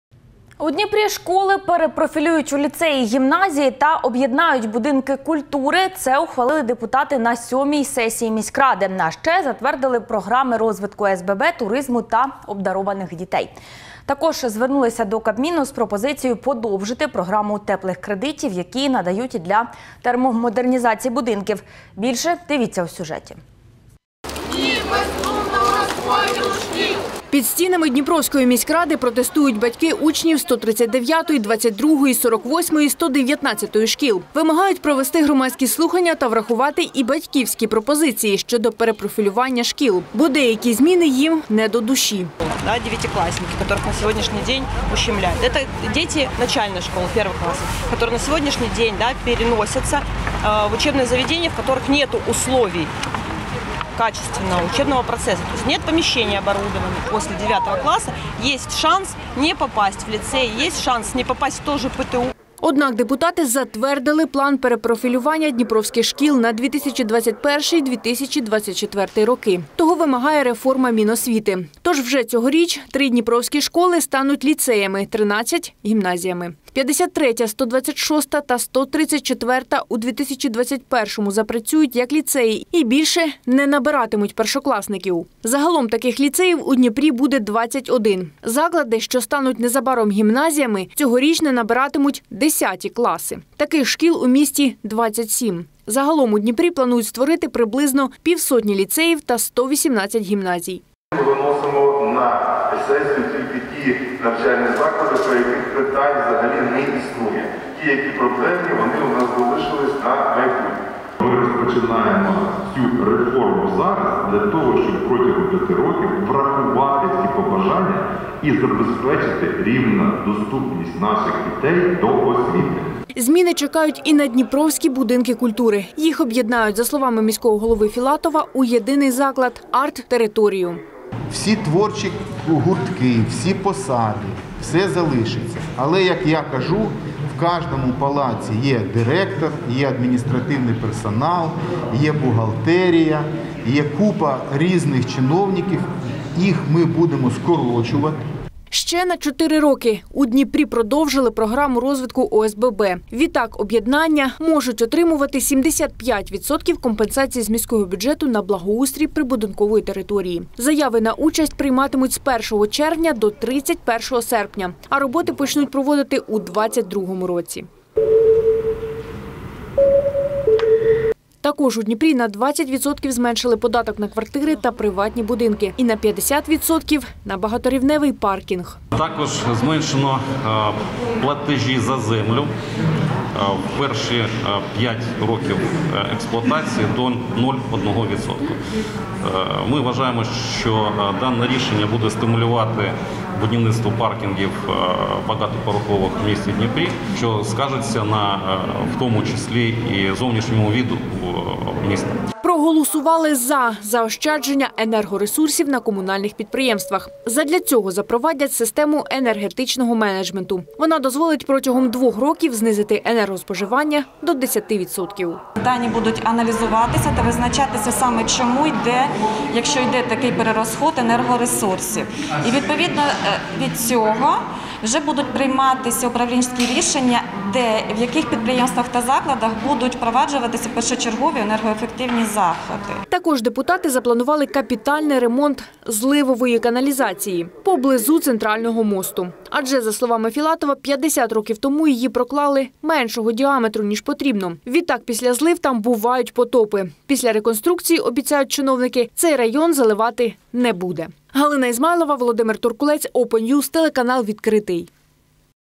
У Дніпрі школи перетворять на ліцеї й гімназії: результати чергової сесії міськради
У Дніпрі школи перепрофілюють у ліцеї й гімназії та об’єднають будинки культури. Це ухвалили депутати на сьомій сесії міськради. А ще затвердили програми розвитку ОСББ, туризму та обдарованих дітей. Також звернулися до Кабміну з пропозицією подовжити програму теплих кредитів, які надають для термомодернізації будинків. _______________________________________________